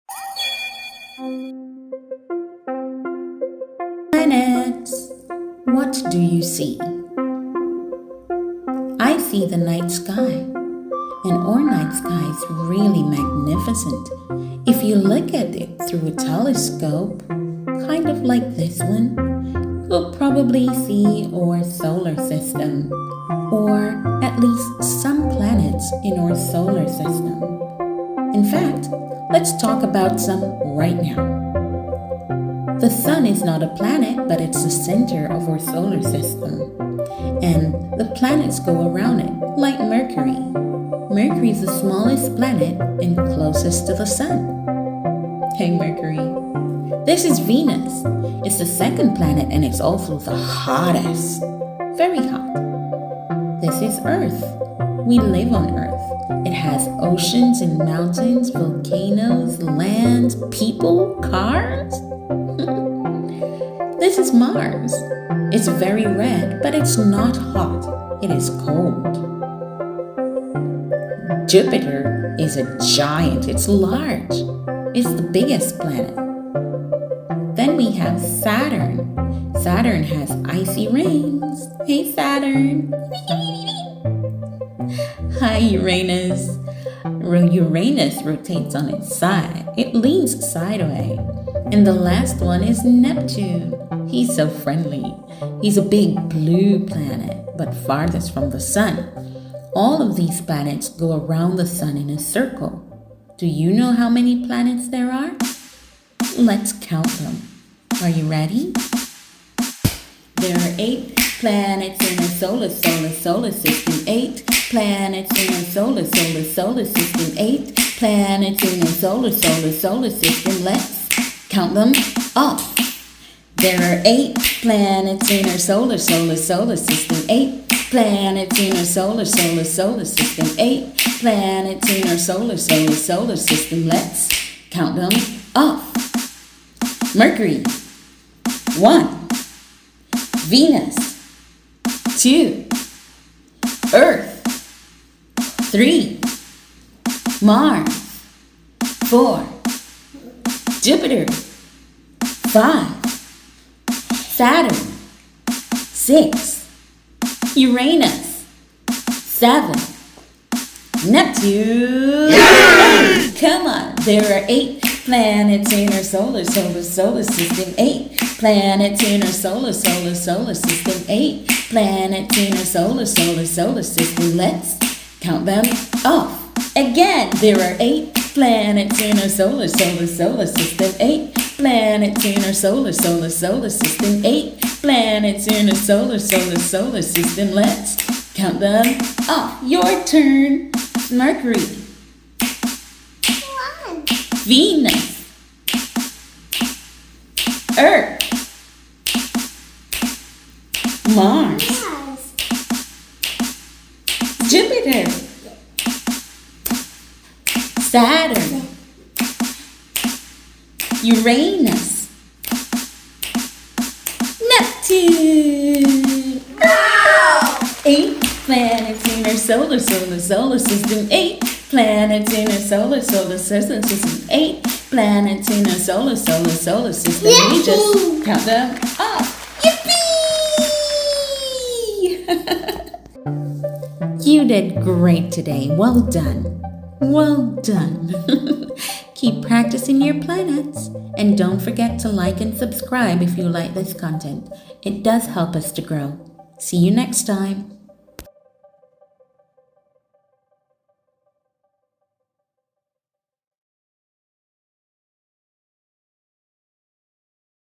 Learn the Planets|Planet Song for Toddlers|counting
In this Learn the Planets video for toddlers, we'll be learning about the planets using song and simple activities. We'll be counting to represent each planet, and singing songs about each one.

If you're looking for an easy way to introduce your toddler to the planets, then this Learning Video is perfect for you! With catchy songs and activities, your little one will learn about the planets in a fun and easy way. Don't miss out - learn the planets with Kiddyreal's Learn the Planets video for toddlers today!
This episode on Kiddyreals is about learning the 8 planets. Help your little kiddys learn the planets and through a catchy little song too.